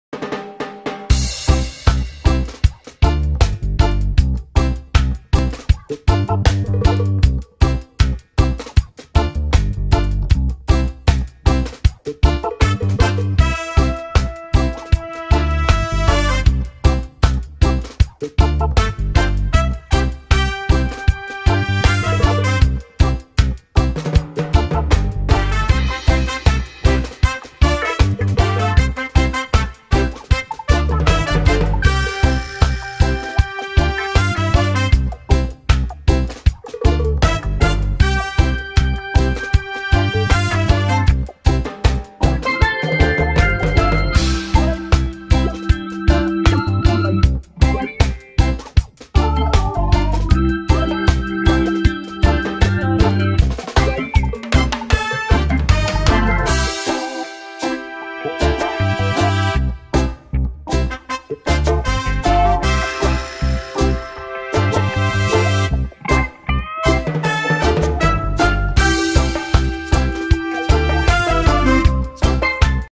Turkish ice-cream man prank with customer।। customer Enjoy with Turkish ice-cream
Turkish ice-cream man praknk with customer।। customer Enjoy with Turkish ice-cream ..